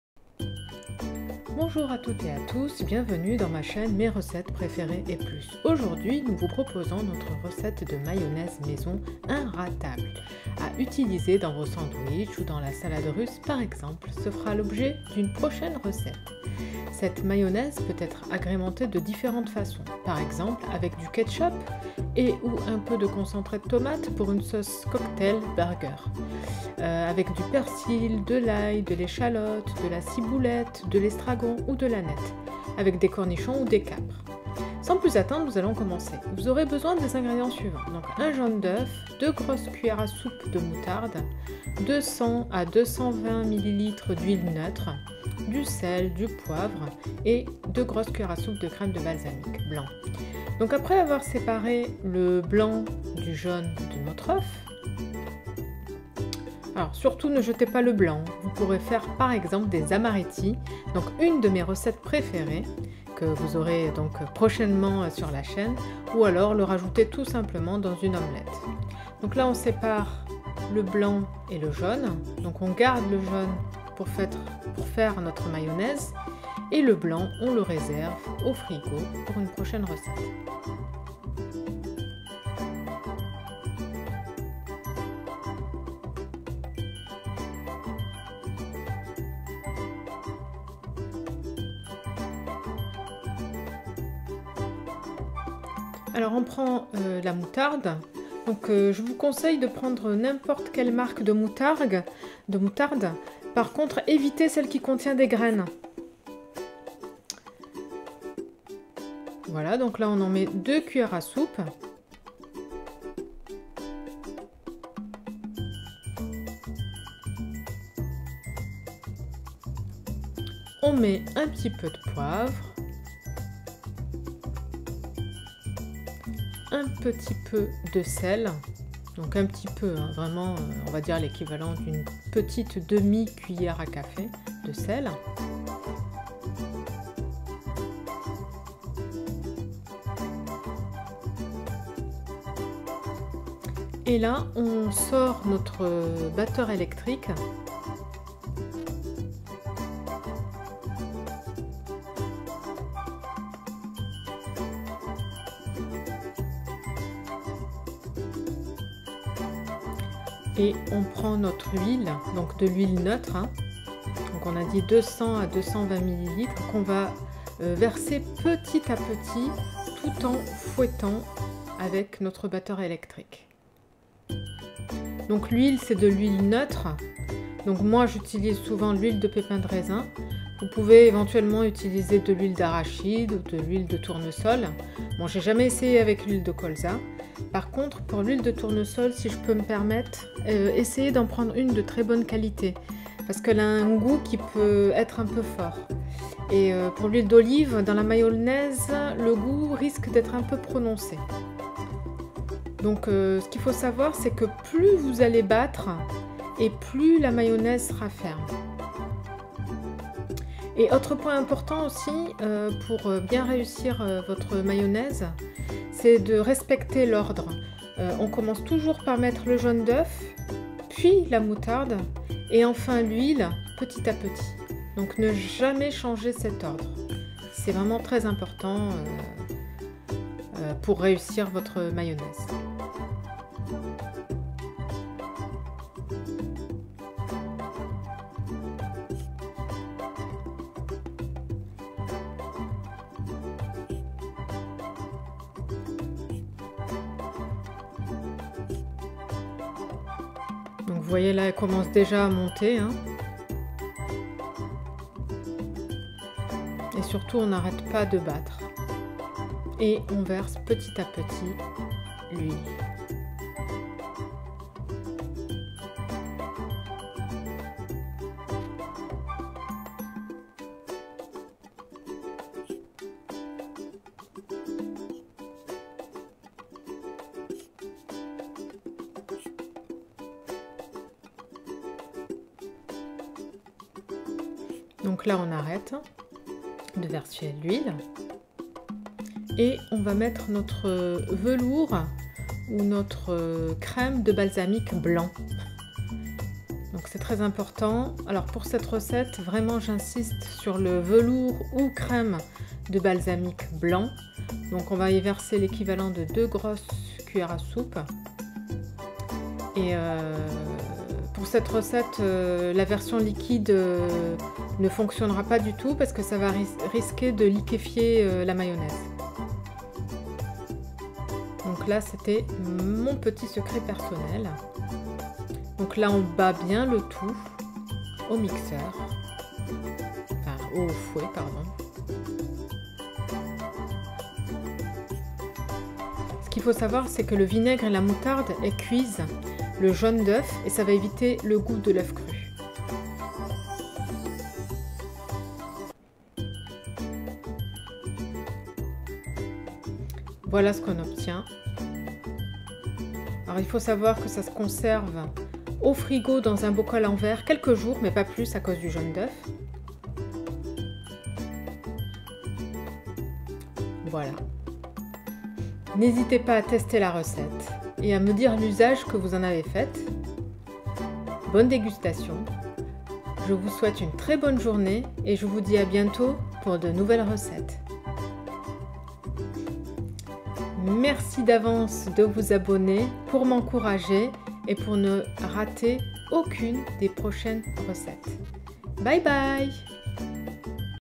Recette de MAYONNAISE maison inratable et facile 👍
Bienvenue dans ma chaîne Mes recettes préférées &+. Aujourd'hui, je vous propose notre recette de mayonnaise maison, in ra table, facile et délicieuse, à utiliser dans vos sandwichs ou dans la salade russe, par exemple. Cela fera l'objet d'une prochaine recette.
Cette mayonnaise peut-être agrémentée de différentes façons.Par exemple :
    - Avec du Ketchup et/ou un peu de concentré de tomates pour une sauce cocktail/burger
    - Avec du persil, de l'ail, de l'échalote, de la ciboulette, de l'estragon ou de l'aneth
   - Avec des cornichons ou des câpres.

Ingrédients :
Un jaune d’œuf
2 grosses CS de moutarde
200 à 220 ml d'huile neutre
Sel (1/2 cc) et Poivre à discrétion
Secret personnel : 2 grosses CS de crème de balsamique blanc